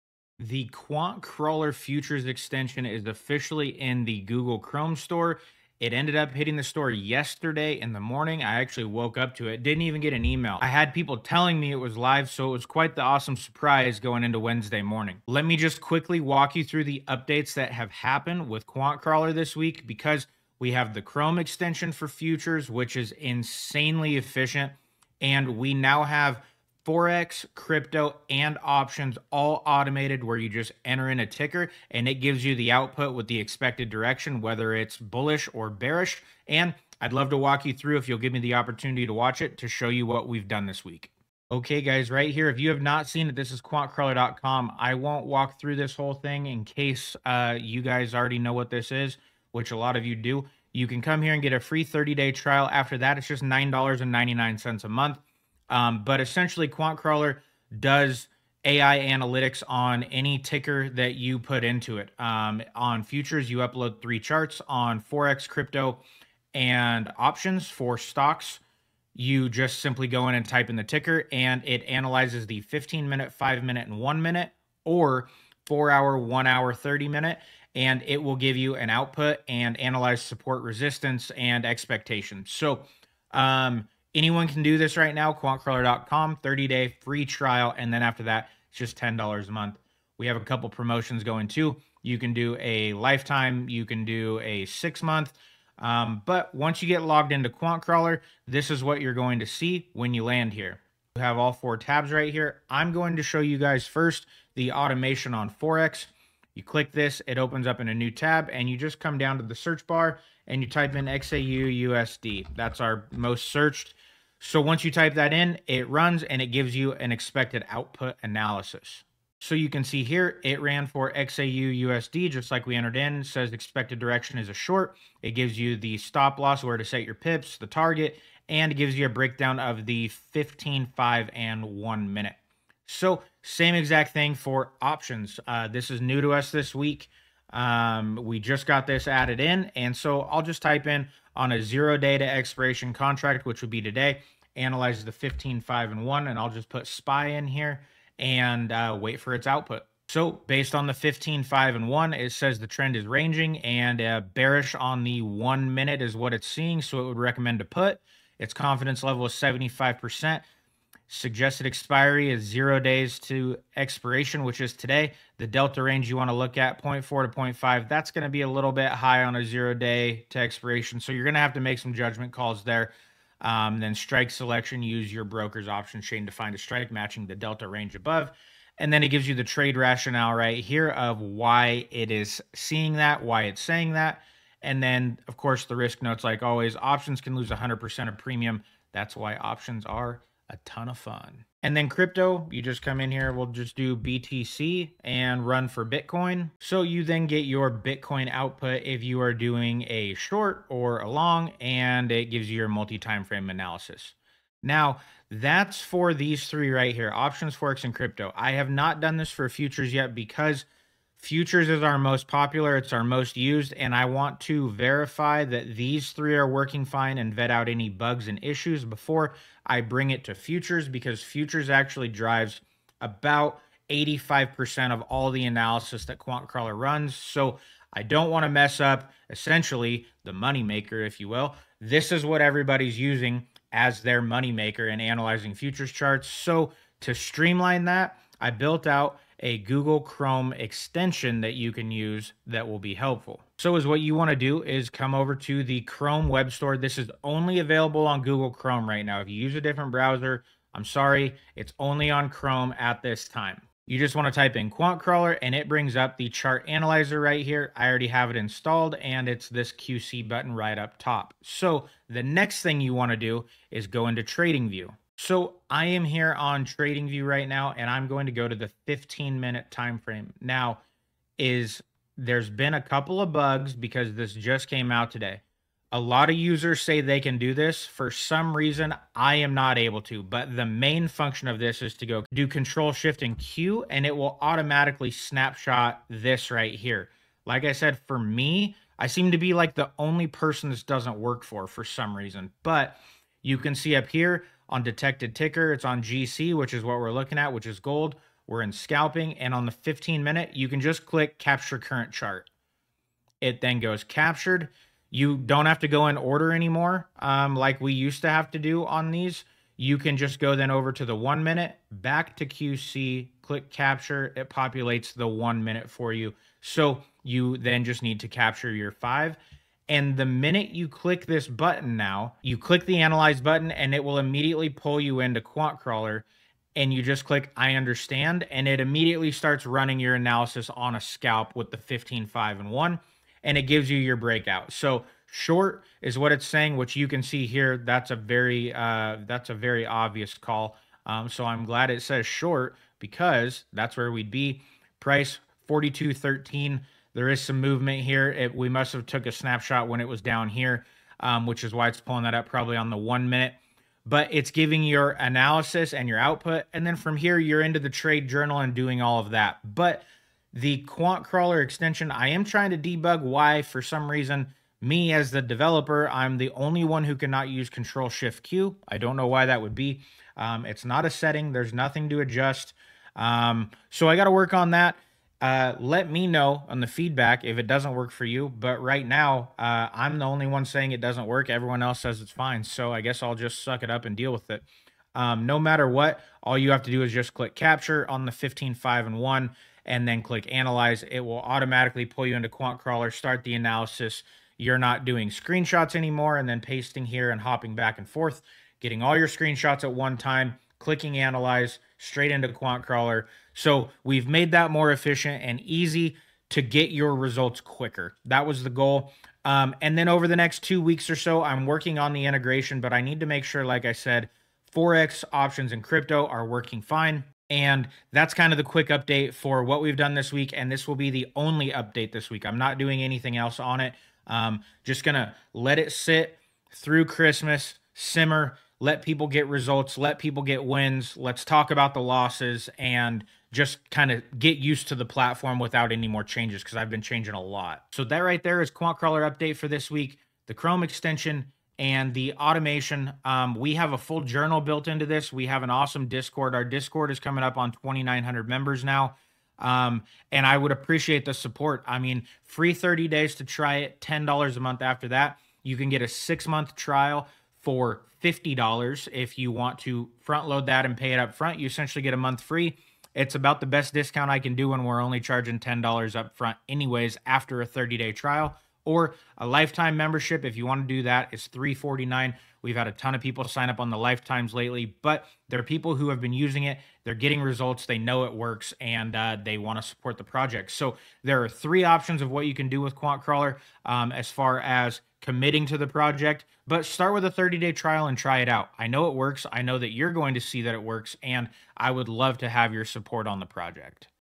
QuantCrawler Futures Extension Just Launched!
QuantCrawler keeps getting faster. This week the new Chrome extension for futures went live in the Google Chrome Store, giving you instant chart capture, instant uploading, and one-click AI analysis. If you trade day trading setups like ORB, FVG, scalping on MES, ES, NQ or GC, this update removes the old screenshot workflow and replaces it with a streamlined automated process.

You also now have full automation for forex, crypto, and options. Type a ticker, get direction (bullish or bearish), risk levels, targets, and a breakdown across multi-timeframe analysis. It cuts out guesswork and reduces hesitation so you can focus on risk, psychology, and execution.

QuantCrawler handles futures chart analysis, multi-timeframe structure, breakout bias, funded account prep, and daily performance tracking. If you want a faster way to analyze markets and tighten your workflow, this update is a solid step forward.

Recommended Tools

QuantCrawler Tools

=== HASHTAGS ===